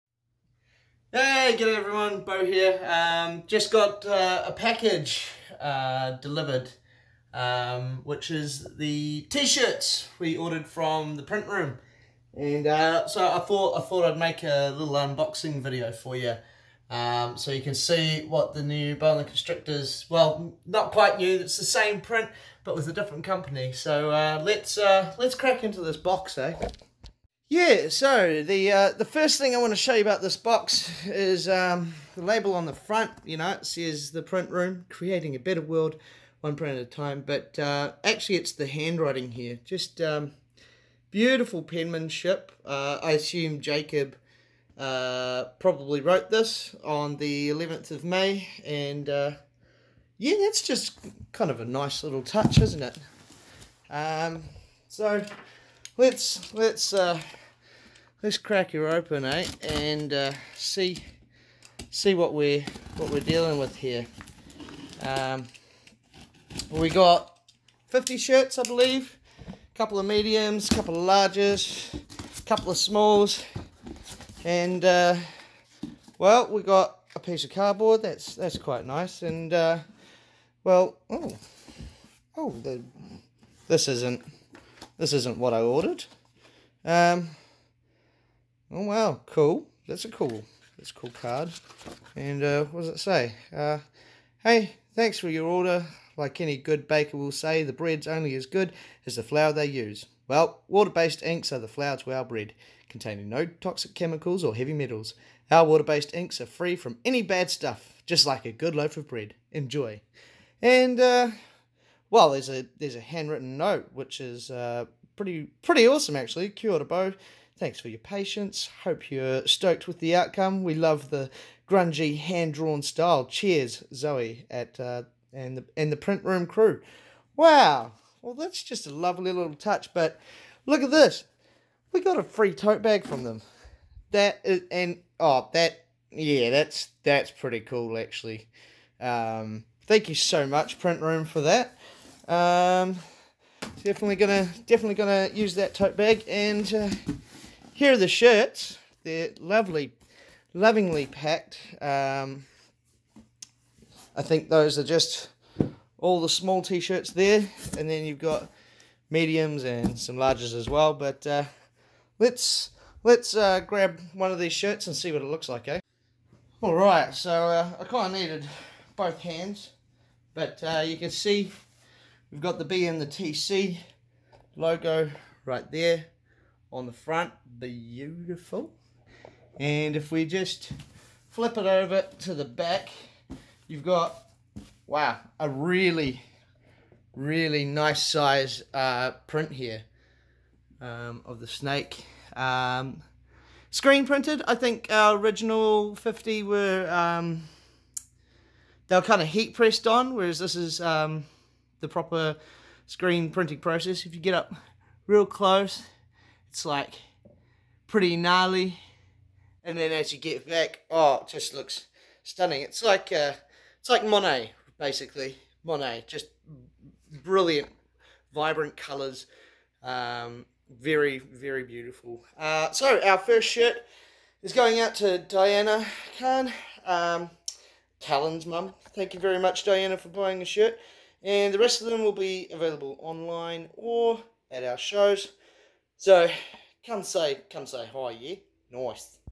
B&TC T SHIRT UNBOXING
Here is Bo with an unboxing video, and showing how good the service is as The Print Room

Hit the shop button on our page to order yourself some merch
Or you can grab one at our future shows!